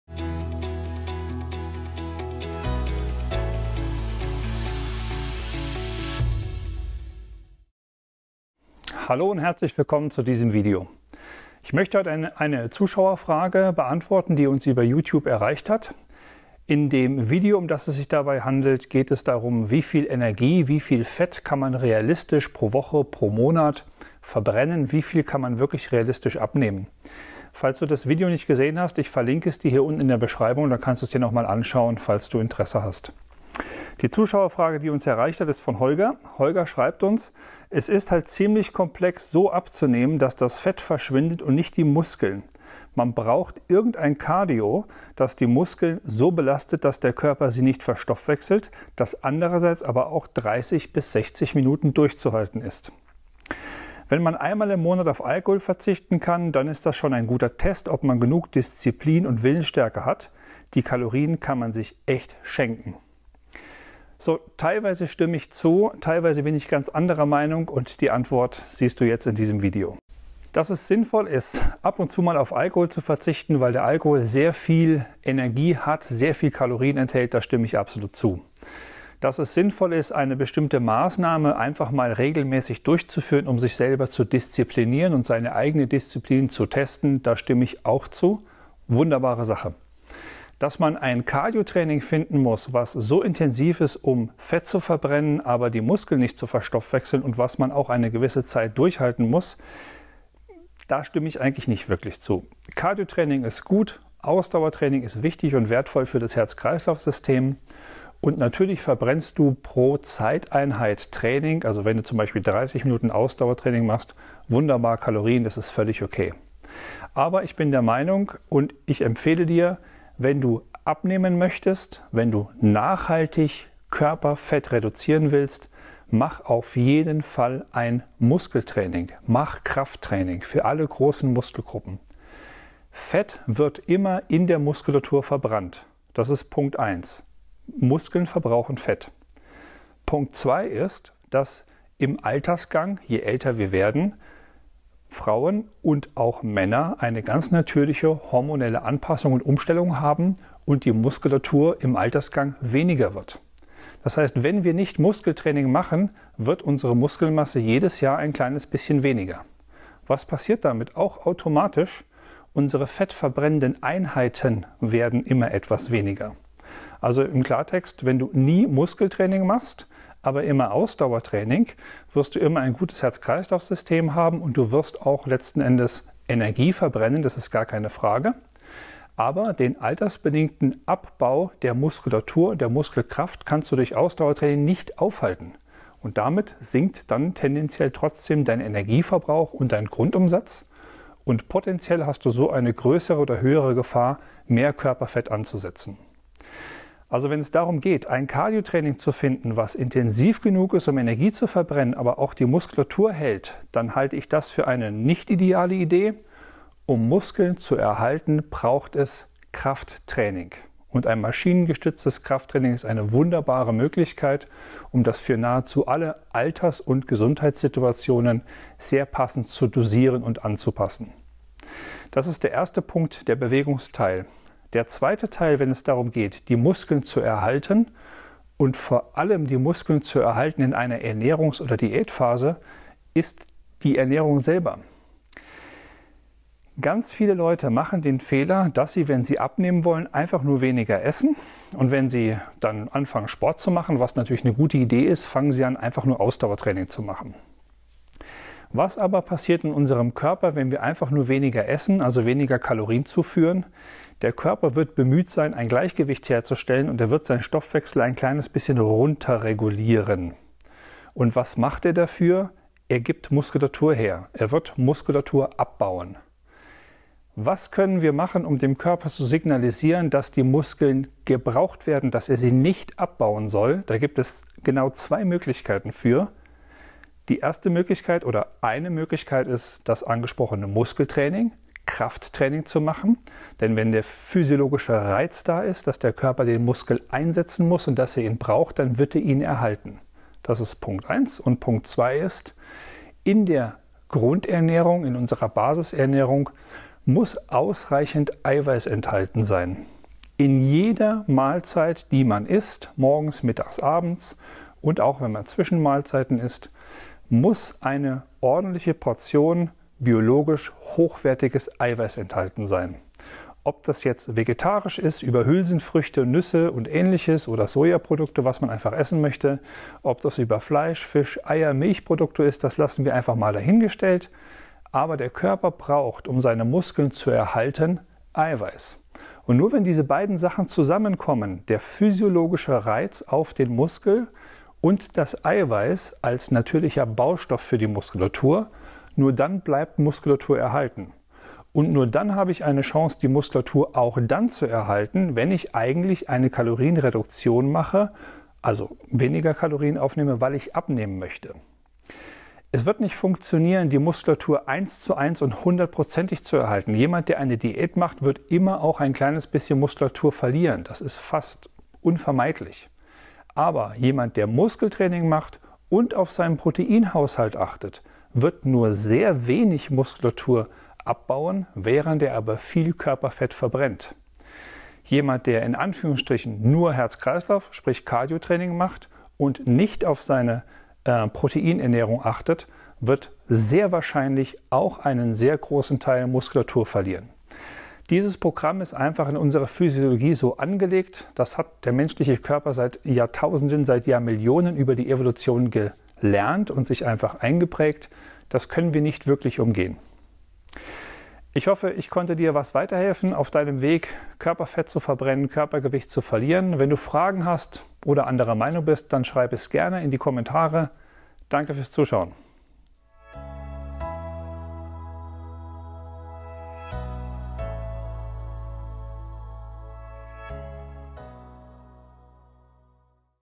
Wir beantworten eine Zuschauerfrage: Mit Ausdauertraining Muskeln aufbauen und erhalten? Geht das?
Ist es möglich seine Muskeln mit Ausdauertraining aufzubauen? In diesem Video beantworten wir eine Zuschauerfrage zum Thema Abnahmen bzw. Kalorienverbrauch und Ausdauertraining.